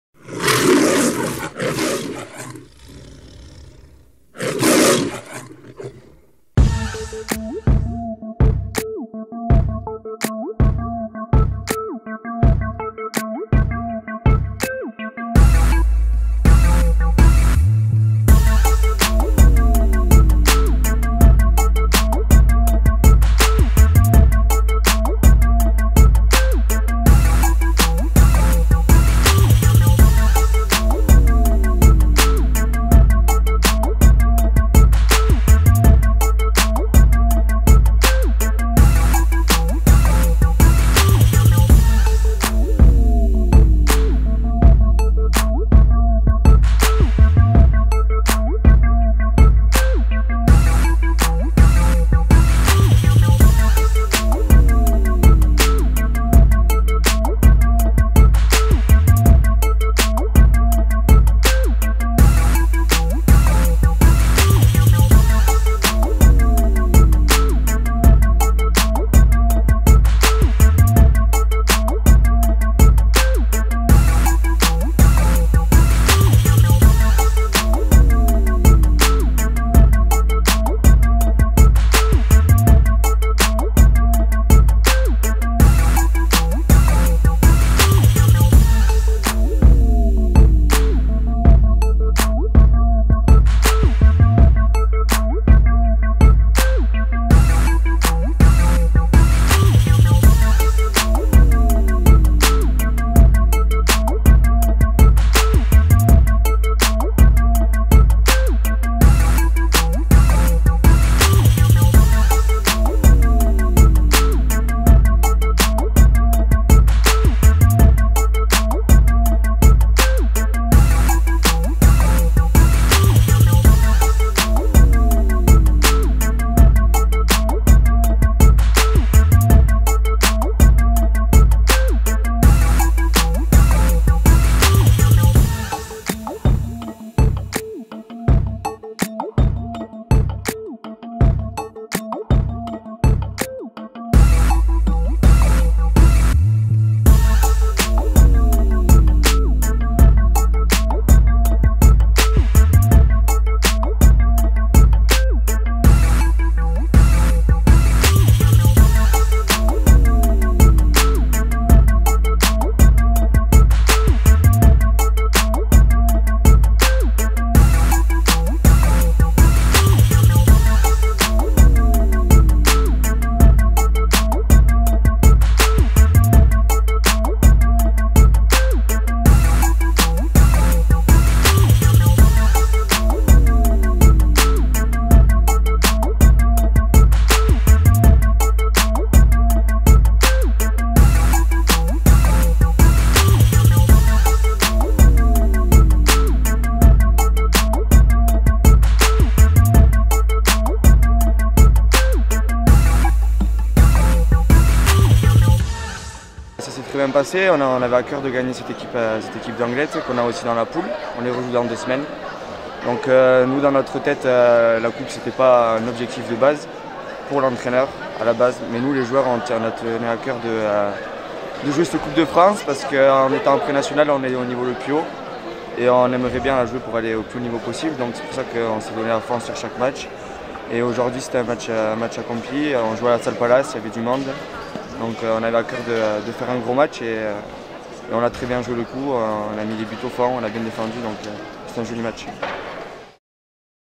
Handball masculin : HBCO-Anglet
L'équipe seniors du HBCO recevait Anglet en coupe de France de Handball.
Victoire du HBCO 31-27.